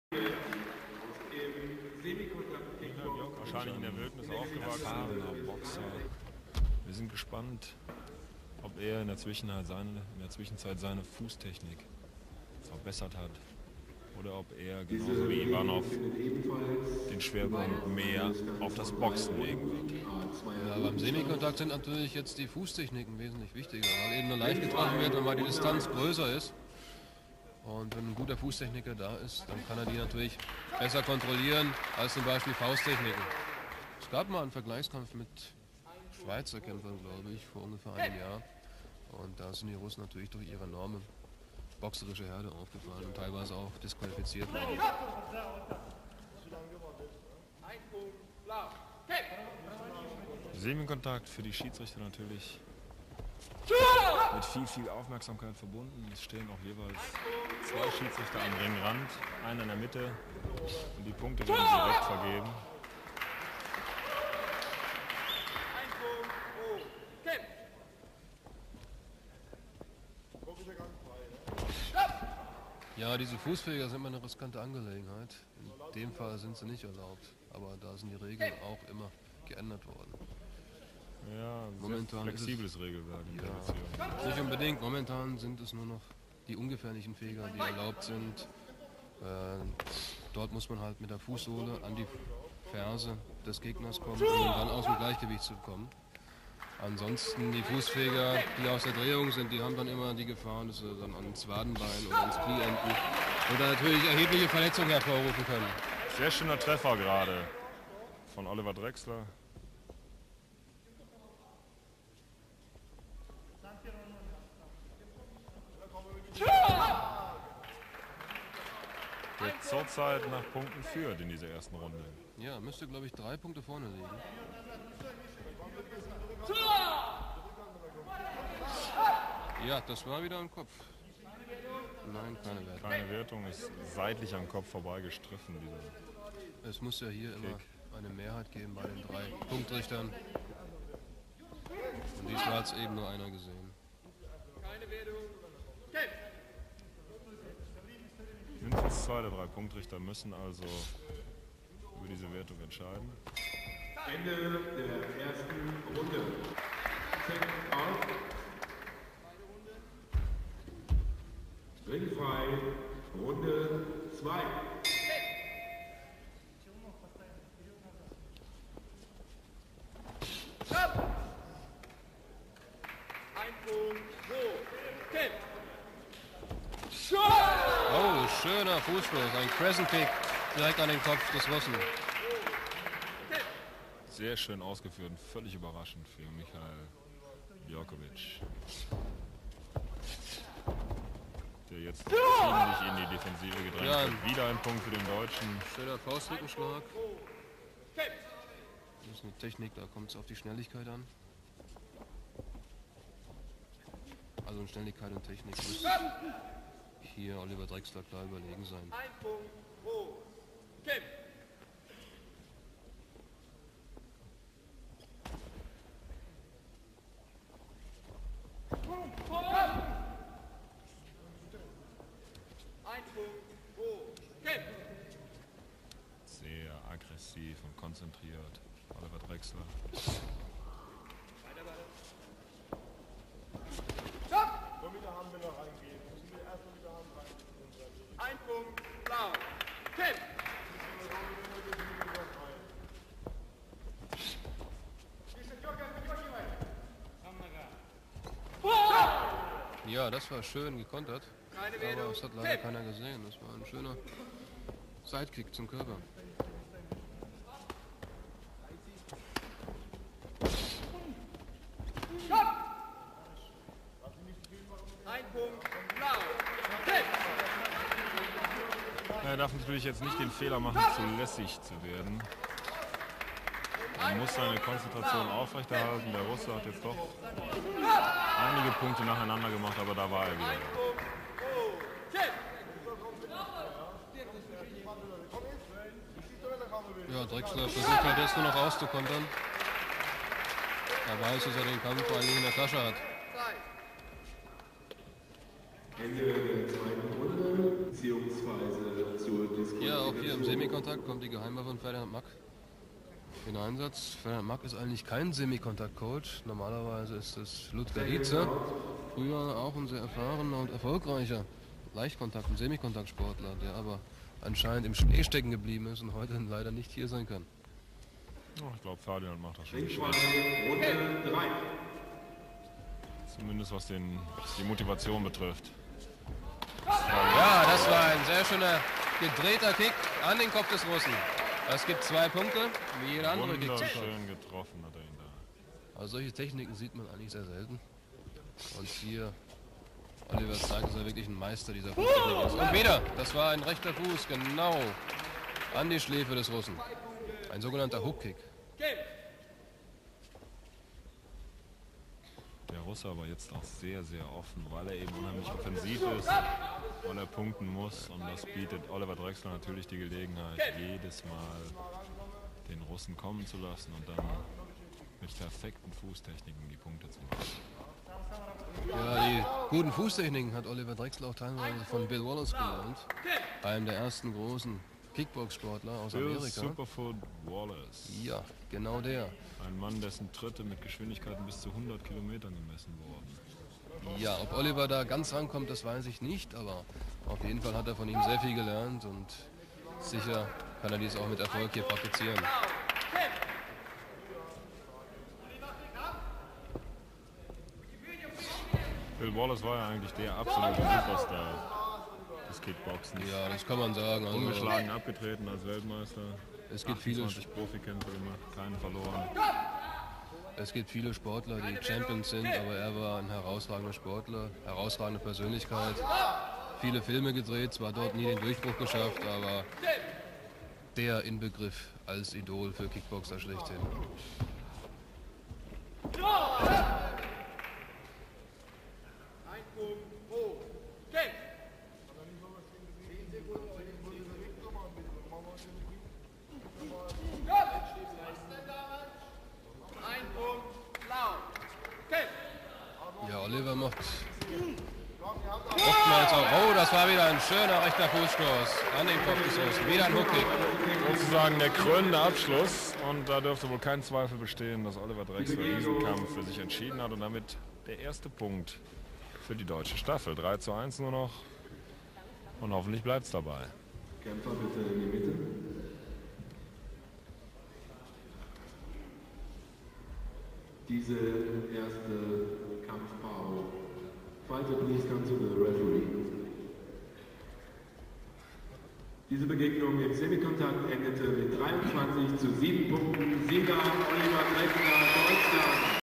Oliver Drexler, Semikontakt Kickboxen
10. Februar 1991
Oliver Drexler kämpft für Deutschland im ersten Teamfight gegen All Eurasiatische/Russische Auswahl. Erstmals russische Fighter in Deutschland.